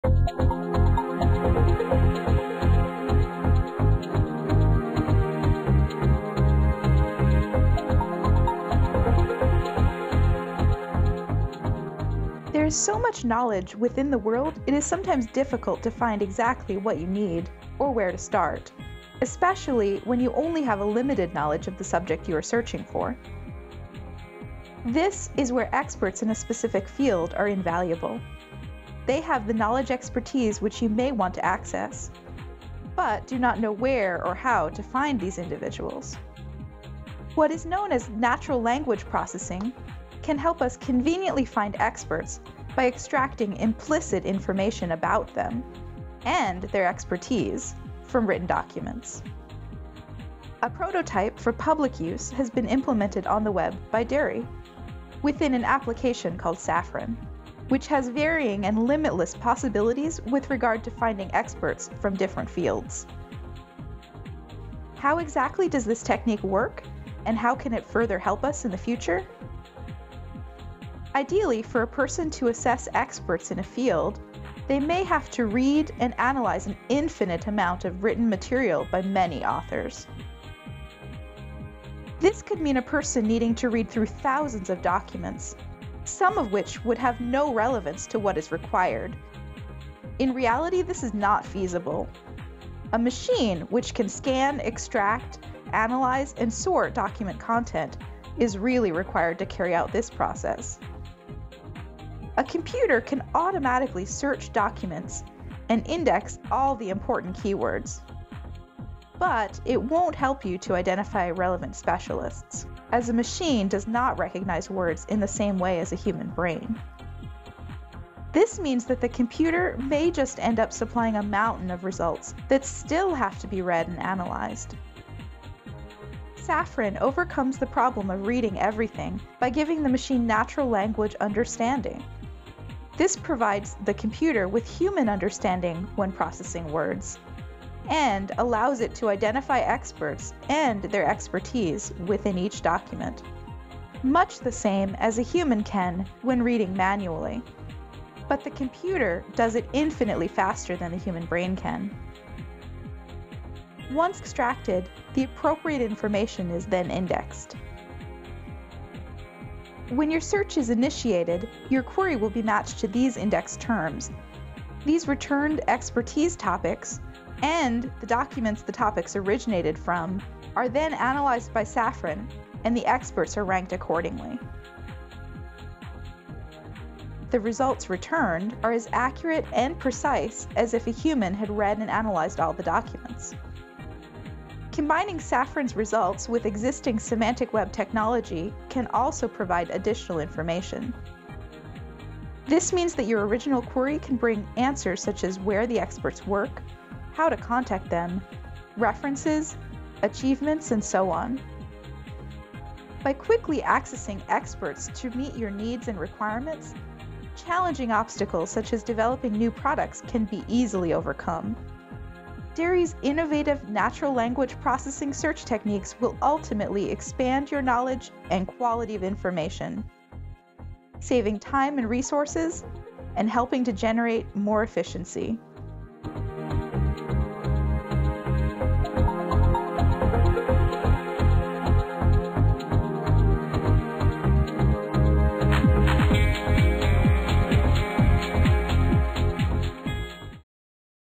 Natural Language Processing Explained - Expert Finding with Saffron
This video is one of a series which explains some of the research being carried out at DERI, NUI Galway.

This video explains Natural Language Processing and its use in extracting specific data for expert finding.